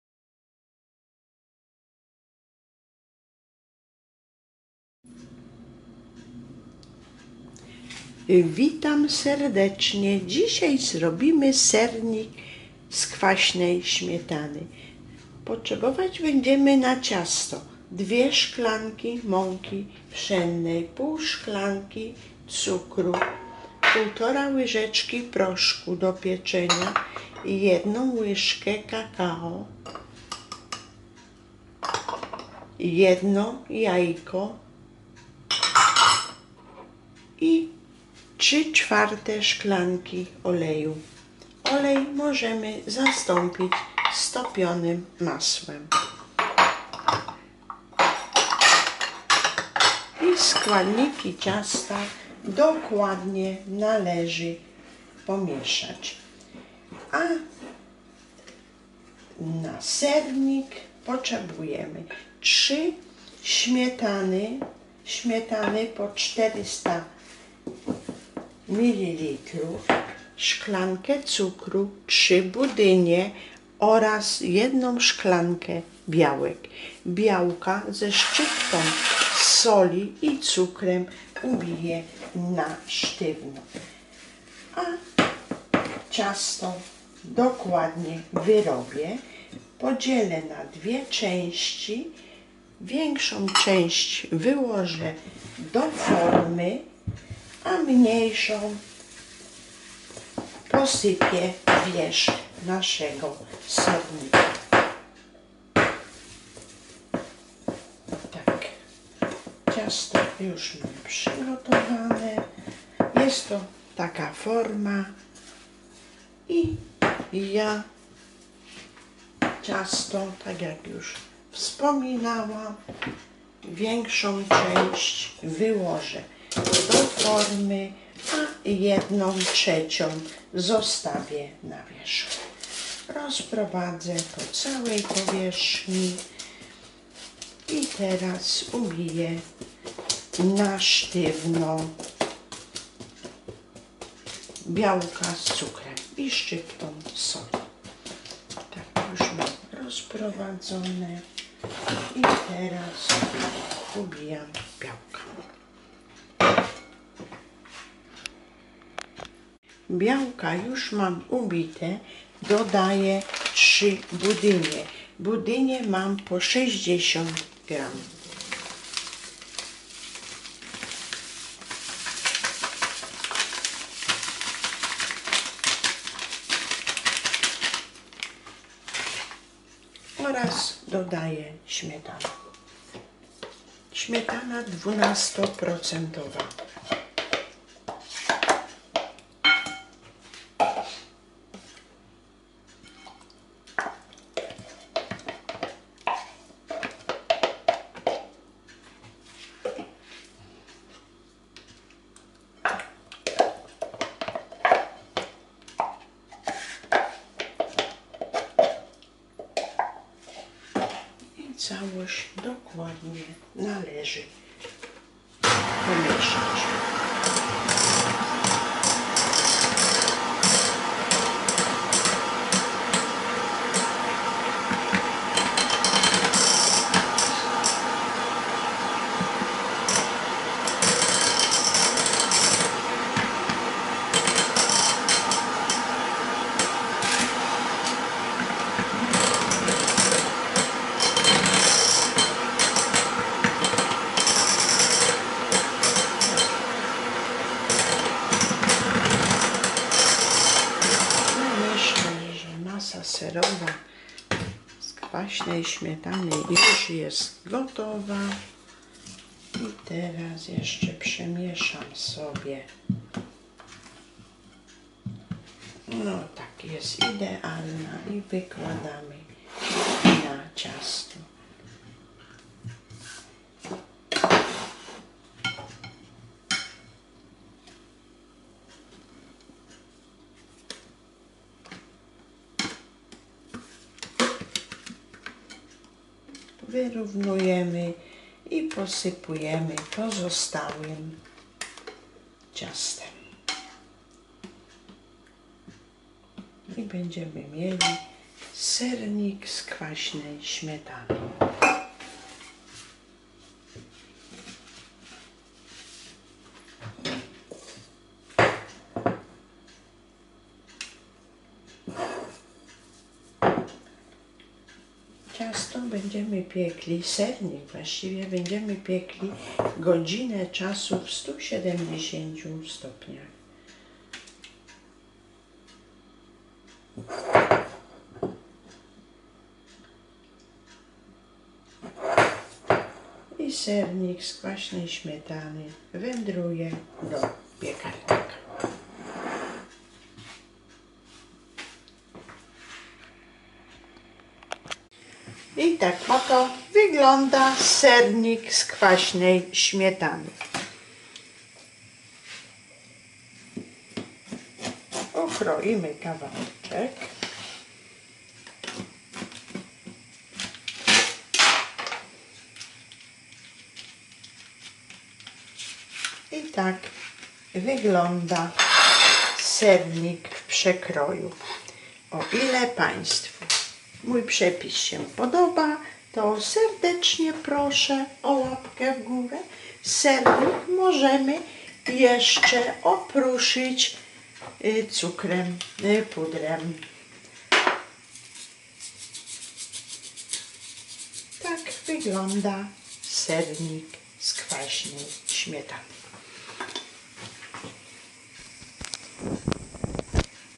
Sernik z kwaśnej śmietany, pyszny i puszysty. Prosty przepis jak zrobić smaczny i łatwy sernik
Dziękuję !! heniafoks heniafoksciasta heniafoksprzepisy Jak upiec łatwy i pyszny sernik? Przepis babci jak zrobić smaczny i puszysty sernik z kwaśnej śmietany.

Składniki:
2 szklanki mąki
1 jajko
1/2 szklanki cukru
1,5 łyżeczki proszku do pieczenia
3/4 szklanki oleju
1 łyżka kakao
szczypta soli

Ser
1 szklanka białek
3 szt, kwaśnej śmietany 12 % [400 ml]
3 budynie 60 g
1 szklanka cukru

*szklanka o pojemności 250 ml

Sprawdzone, tradycyjne, domowe przepisy w nowoczesnym wydaniu.